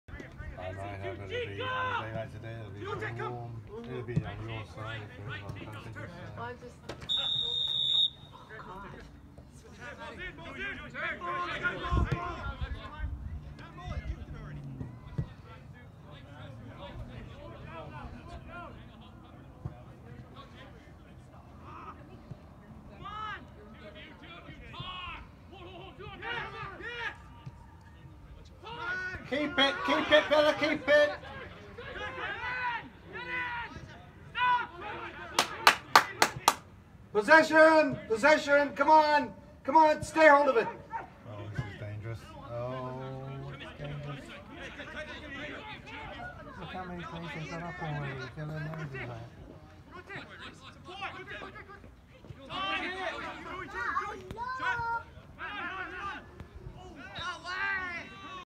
Powell River Villa (red) versus Westcastle United at Timberlane Park February 8, 2020
Powell River Villa (red) hosted Westcastle United for a Vancouver Island Soccer League Division 1 game at Timberlane Park on Saturday, February 8, 2020. Here is a clip of the action.